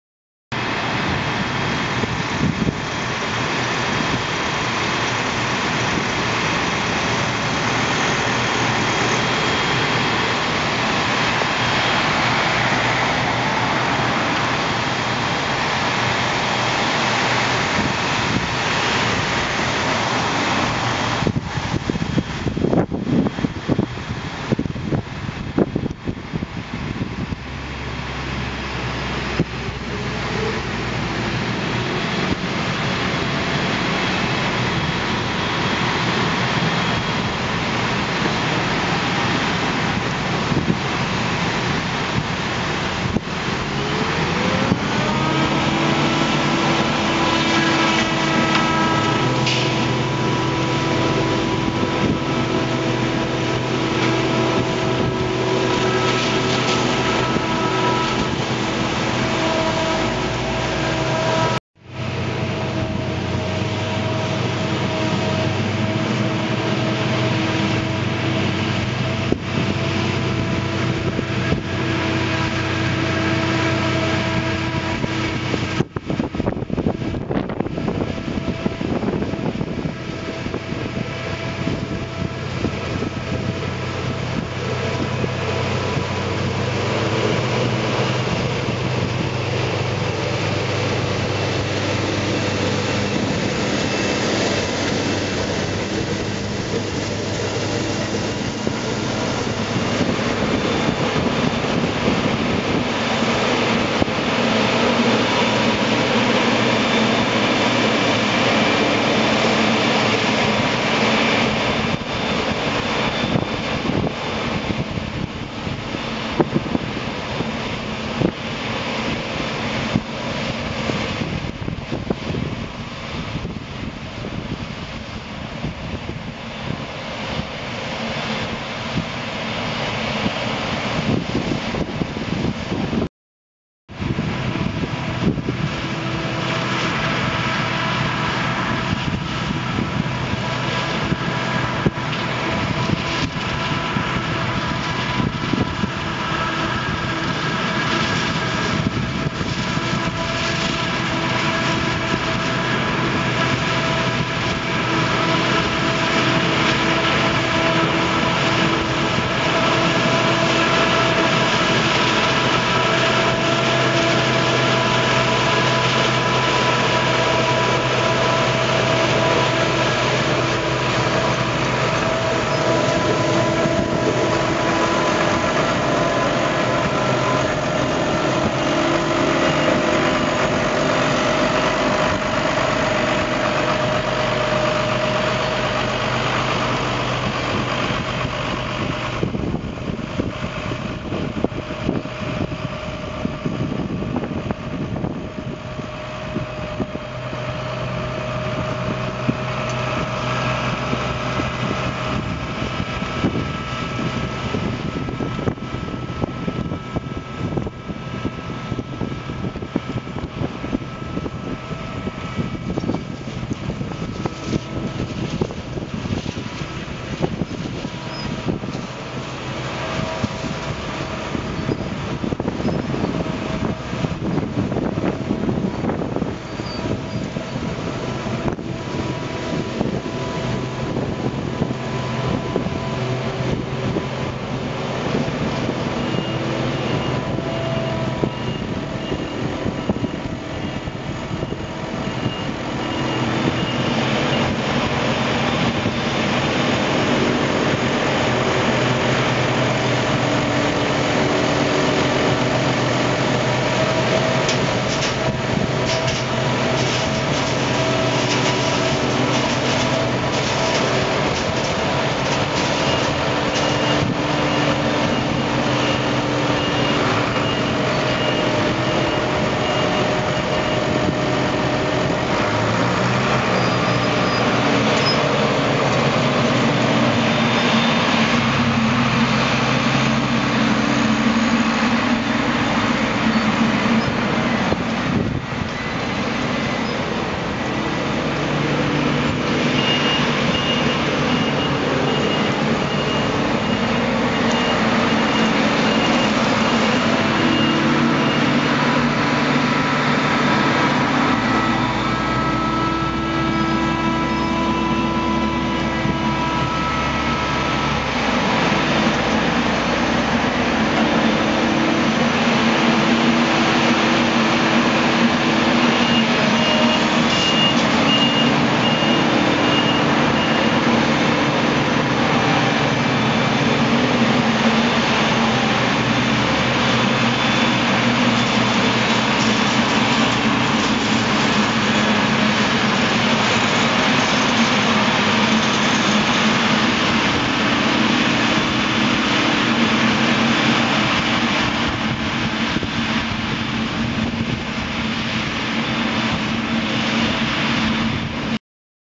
Ooms mais hakselen 2016 (2) , Claas Jaguar 960
Loonwerken Ooms uit Brecht mais hakselen 2016 met hun Claas Jaguar 960 en bijgestaan door ,
- New Holland T 7050 met Record tractie kipper
- New Holland TVT 155 met nieuwe Claas dubbeldoel Cargos 8500
- New Holland TVT 170 met Ipsam 3 - asser